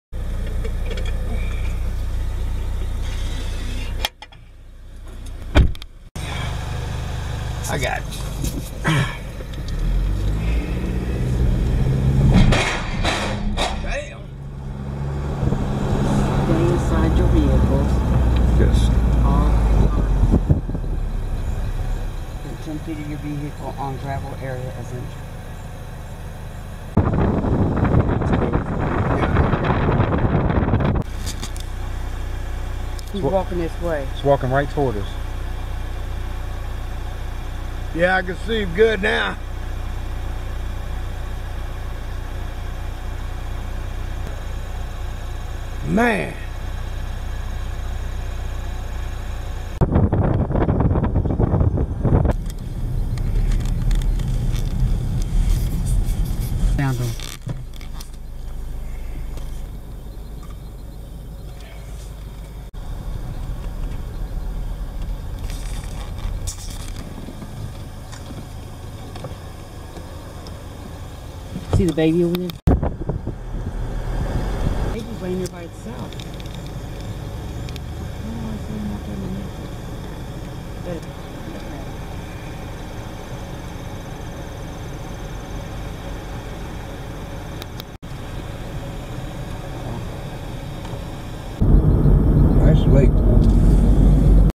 Elk and Bison Prairie - Land Between the Lakes
A short video of the drive through the Elk and Bison Prairie at Land Between the Lakes.